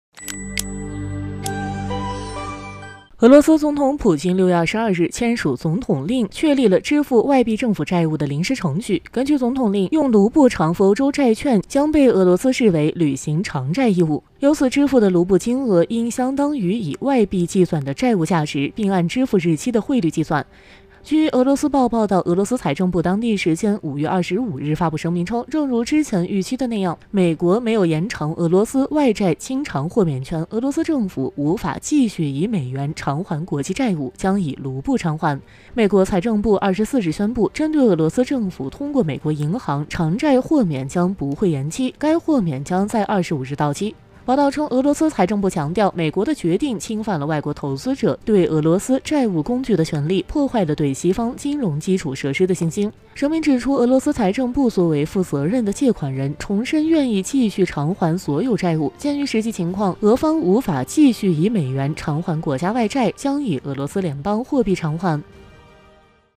确定了！普京正式签署总统令，俄罗斯将用卢布偿还外债
确定了！普京正式签署总统令，俄罗斯将用卢布偿还外债OK! Putin formally signed a presidential decree that Russia will repay its foreign debt in rubles